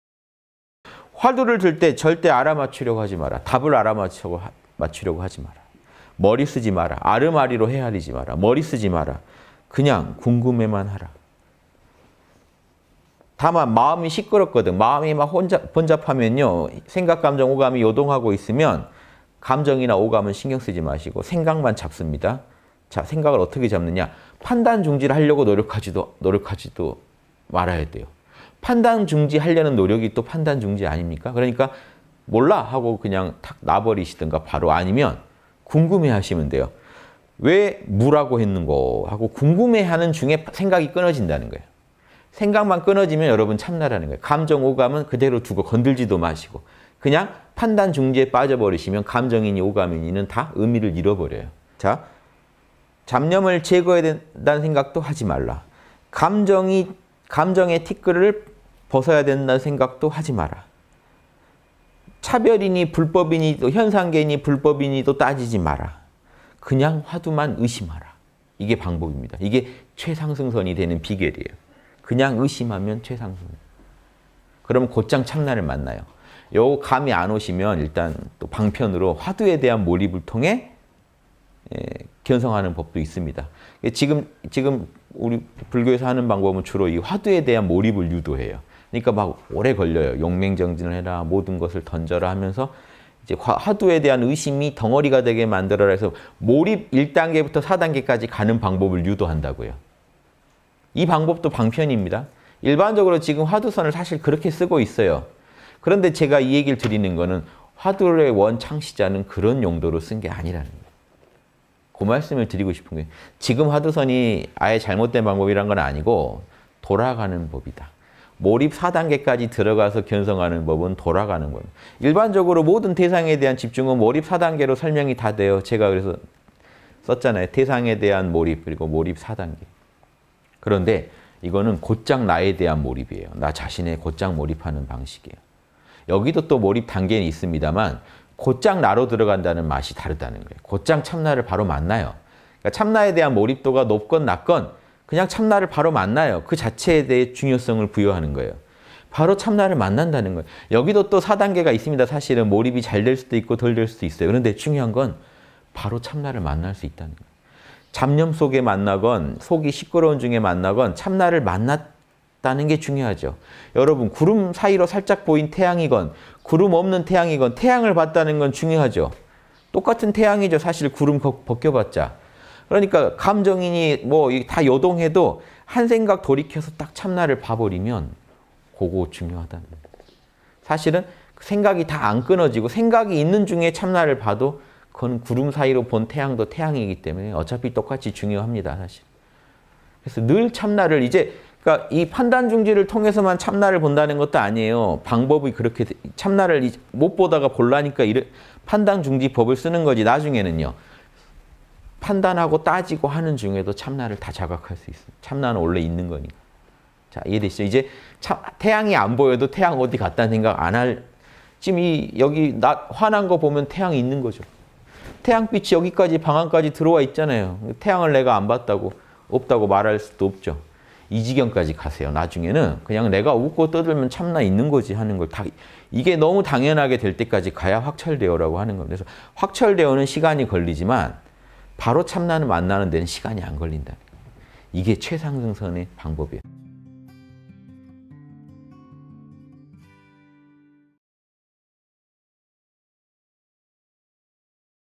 [3분 법문] 실전 화두선- 그냥 의심하라!_홍익선원.윤홍식
(170601 목낮대담) "화두를 들 때 절대 알아맞히려고 하지 마라!"
"답을 알아맞히려고 하지 마라!"
"머리 쓰지 마라!"
"알음알이로 헤아리지 마라!"
"머리 쓰지 마라!"
"그냥 궁금해만 하라!"
다만 마음이 시끄럽거든,
마음이 막 번잡하면, 생각⋅감정⋅오감이 요동하고 있으면,
감정이나 오감은 신경 쓰지 마시고 생각만 잡습니다.

자, 생각을 어떻게 잡느냐?
판단 중지를 하려고 노력하지도 말아야 돼요.
판단 중지하려는 노력이 또 판단 아닙니까?
그러니까 "몰라!" 하고 그냥 탁 놔 버리시든가
바로 아니면 궁금해하시면 돼요.

"왜 무(無)라고 했는고?" 하고 궁금해하는 중에 생각이
끊어진다는 거예요.
생각만 끊어지면 여러분 참나라는 거예요.

감정⋅오감을 그대로 두고 건드리지도 마시고,
그냥 판단 중지에 빠져 버리시면,
감정이니 오감이니는 다 의미를 잃어버려요.
자, "잡념을 제거해야 된다는 생각도 하지 마라!"
"감정의 티끌을 벗어야 된다는 생각도 하지 마라!"
"차별이니 불법이니 현상계니 불법이니도 따지지 마라!"
"그냥 화두만 의심하라!"
이게 방법입니다.
이게 최상승선이 되는 비결이에요.
그냥 의심하면, 최상승선이에요. 그러면 곧장 참나를 만나요.

이 감이 안 오시면,
일단 또 방편으로 화두에 대한 몰입을 통해 견성하는
법도 있습니다.
지금 우리 불교에서 하는 방법은 주로 이 화두에 대한
몰입을 유도해요.
그러니까 막 오래 걸려요.

"용맹정진해라!"
"모든 것을 던져라!" 하면서
"이제 화두에 대한 의심이 덩어리가 되게 만들어라!" 해서
몰입 1단계부터 4단계까지 가는 방법을 유도한다고요.
이 방법도 방편입니다.

일반적으로 지금 화두선을 사실 그렇게 쓰고 있어요.
그런데 제가 이 얘기를 드리는 것은,
화두의 원 창시자는 그런 용도로 쓴 게 아니라는 거예요.
그 말씀을 드리고 싶은 거예요.

지금 화두선이 아예 잘못된 방법이라는 건 아니고,
돌아가는 법이다.
몰입 4단계까지 들어가서 견성하는 법은 돌아가는 법이에요.
일반적으로 모든 대상에 대한 집중은 몰입 4단계로
설명이 다 돼요.

제가 그래서 썼잖아요.
"대상에 대한 몰입, 그리고 몰입 4단계"
그런데 이것은 곧장 나에 대한 몰입이에요.
나 자신에 곧장 몰입하는 방식이에요.
여기도 또 몰입 단계는 있습니다만,
곧장 나로 들어간다는 맛이 다르다는 거예요.
곧장 참나를 바로 만나요.

참나에 대한 몰입도가 높건 낮건 그냥 참나를 바로 만나요.
그 자체에 대해 중요성을 부여하는 거예요.
바로 참나를 만난다는 건, 여기도 또 4단계가 있습니다, 사실은.
몰입이 잘 될 수도, 덜 될 수도 있어요.

그런데 중요한 건 바로 참나를 만날 수 있다는 거예요.
잡념 속에 만나건,
속이 시끄러운 중에 만나건, 참나를 만났다는 게 중요하죠.
여러분 구름 사이로 살짝 보인 태양이건,
구름 없는 태양이건,
태양을 봤다는 건 중요하지요.
똑같은 태양이지요, 사실 구름 벗겨봤자.
그러니까 감정이니 뭐니 다 요동해도 한 생각 돌이켜서,
딱 참나를 봐 버리면 그게 중요하다.

사실은 생각이 다 안 끊어지고 생각이 있는 중에 참나를 봐도,
그건 구름 사이로 본 태양도 태양이기 때문에 어차피 똑같이
중요합니다, 사실.

그래서 늘 참나를,
이제 그러니까 이 판단 중지를 통해서만 참나를 본다는
것도 아니에요.
방법이 그렇게 참나를 못 보다가 보려니까
판단 중지법을 쓰는 것이지 나중에는요?
판단하고 따지고 하는 중에도 참나를 다 자각할 수 있어요.
참나는 원래 있는 것이니까요.

자, 이해되시죠?
이제 태양이 안 보여도 태양이 어디 갔다는 생각 안 하겠죠?
지금 여기 낮 환한 걸 보면 태양이 있는 것이죠.
태양 빛이 여기까지, 방안까지 들어와 있잖아요.
태양을 내가 안 봤다고 없다고 말할 수도 없죠.
이 지경까지 가세요.

나중에는 그냥 "내가 웃고 떠들면 참나 있는 것이지." 하는걸.
이게 너무 당연하게 될 때까지 가야 확철대오라고 하는 겁니다.
그래서 확철대오는 시간이 걸리지만,
바로 참나를 만나는 데에는 시간이 안 걸린다,
이게 최상승선의 방법이에요.